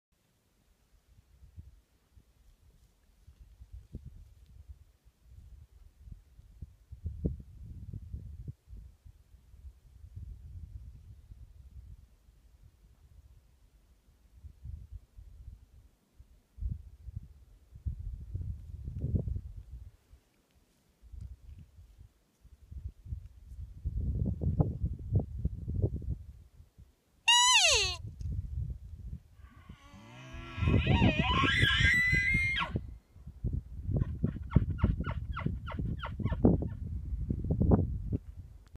Nevada elk bugling
This bull elk was called in from the tree line across the valley below, approximately a mile away and a thousand feet down in elevation.  A cow call is blown around the 26 second mark and the bull responds back with his bugle.